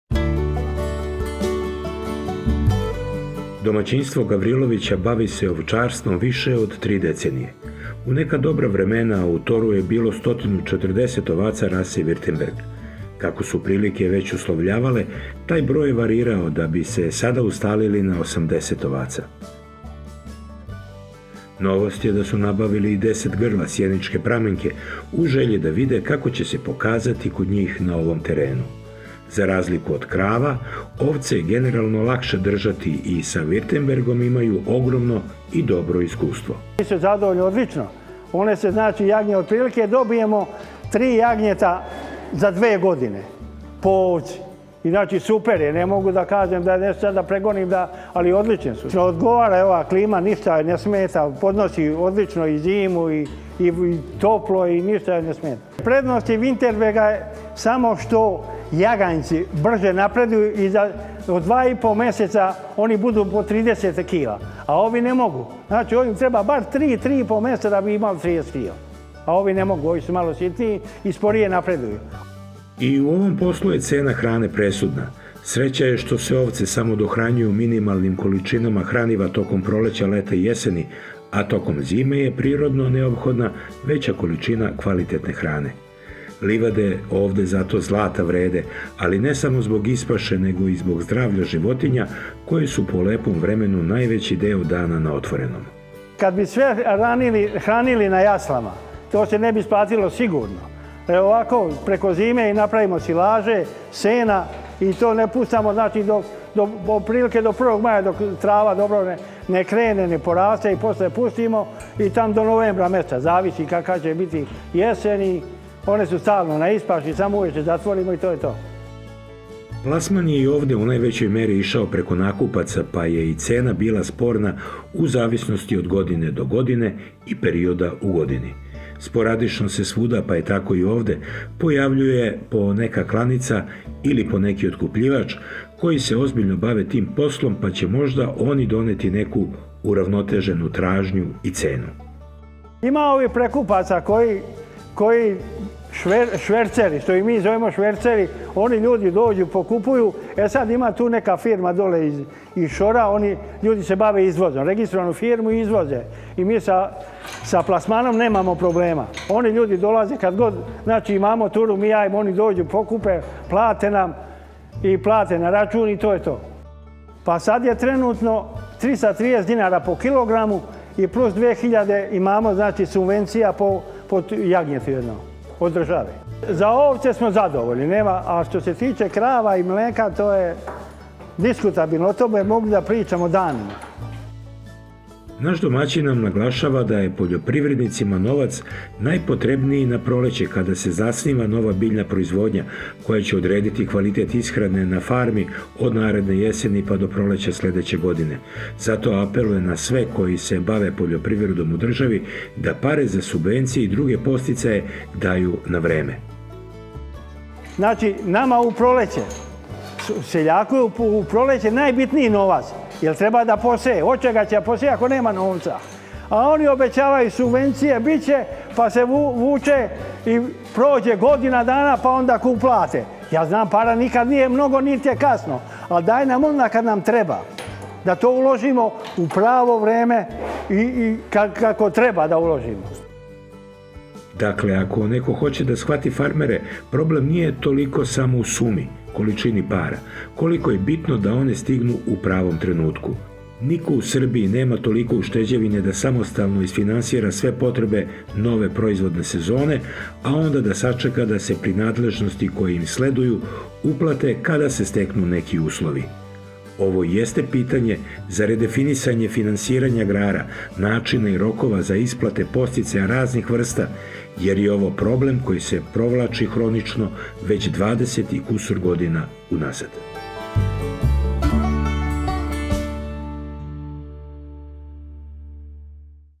U OVČARSTVU je BUDUĆNOST! Farma Gavrilović - Trbosilje
Zbog čega za jednog farmera svetliju budućnost ima ovčarstvo od mlečnog govedarstva?
Sagovornik: Dragorad Gavrilović - Trbosilje
Facebook: Farma emisija o poljoprivredi